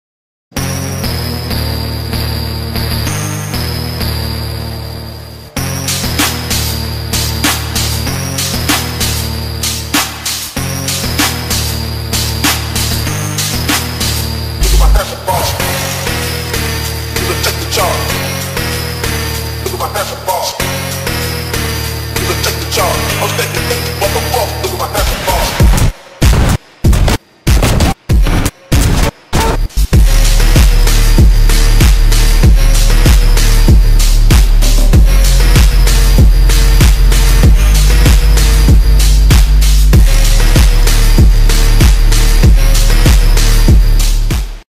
Soviet Union edit, but i try to sync beat with T-80UM2 bounces
I tried man.